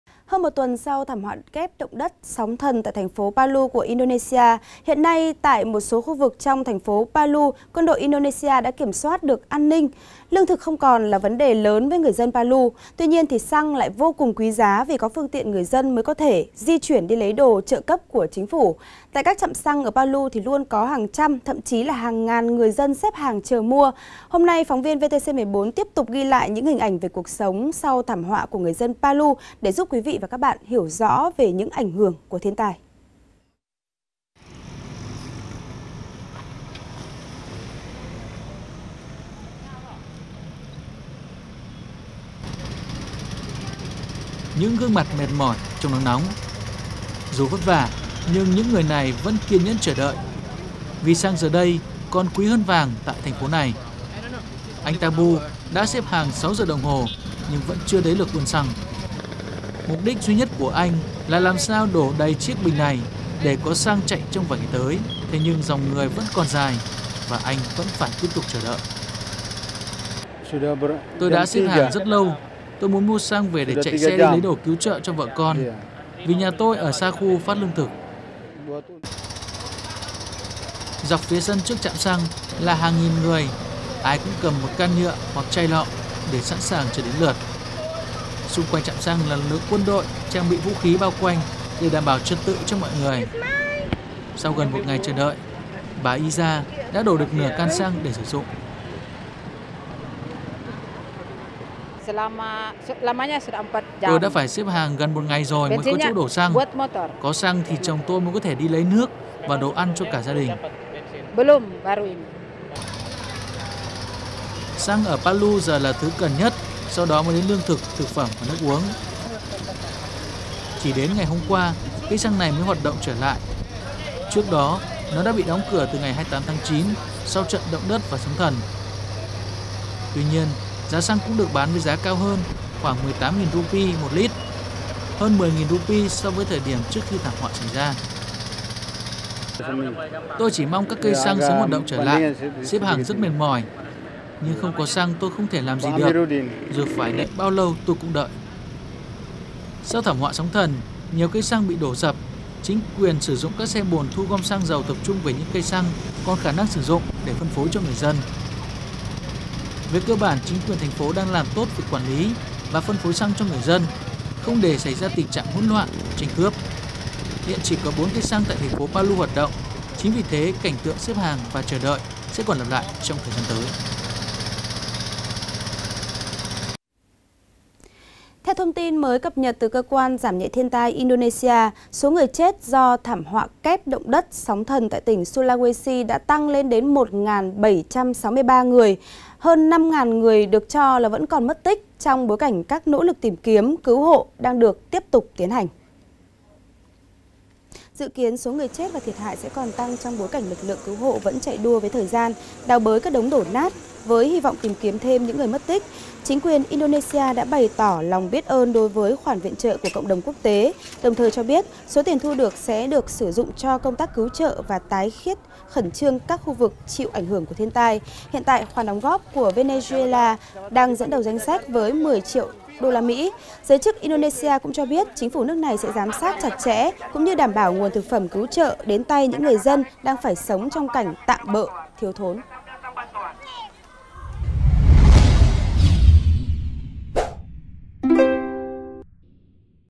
Indonesia: người dân Palu xếp hàng cả ngày để mua xăng| VTC14
VTC14 |INDONESIA: NGƯỜI DÂN PALU XẾP HÀNG CẢ NGÀY ĐỂ MUA XĂNG
Hiện nay tại một số khu vực trong thành phố Palu, Quân đội indonesia đã kiểm soát được an ninh. Lương thực không còn là vấn đề lớn với người dân Palu. Tuy nhiên xăng lại vô cùng quý giá vì có phương tiện người dân mới có thể di chuyển đi lấy đồ trợ cấp của chính phủ.